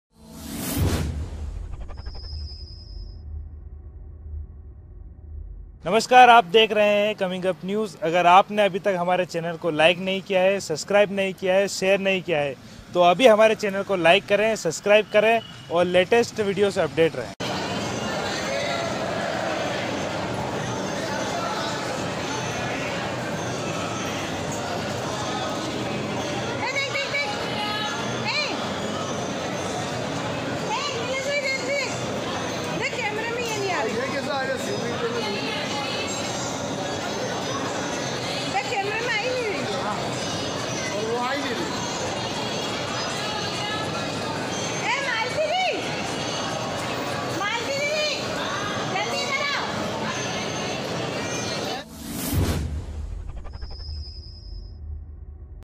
बाबा महाकाल के आंगन में कैमरे में कैद हुए बाबा बफार्नी | साक्षात दिए दर्शन | देखें Live विडियो...
ComingUp News उज्जैन। बाबा महाकाल मंदिर में दर्शन करने आए श्रद्धालू के मोबाईल कैमरे में राजाधिराज की शिवलिंग कैद होने का विडियो वायरल हुआ है। जो कि सोशल मीडिया पर जमकर ट्रोल हो रहा है। विडियो सामने आने के बाद तरह-तरह बातें सामने आ रही है। कोई इसे राजाधिराज के आंगन में बाबा बफार्नी की दस्तक बता रहा है। तो कोई इस भक्तों को बाबा के दर्शन देने की आस्था से जोड़कर देख रहा है। खैर जो भी हो लेकिन विश्व प्रसिद्ध बाबा महाकाल मंदिर में इस प्रकार का विडियो सामने आने के बाद श्रद्धालू इसे आस्था से जोड़कर देख रहे है और वे इसे भक्तों को बाबा के दर्शन मान रहे है।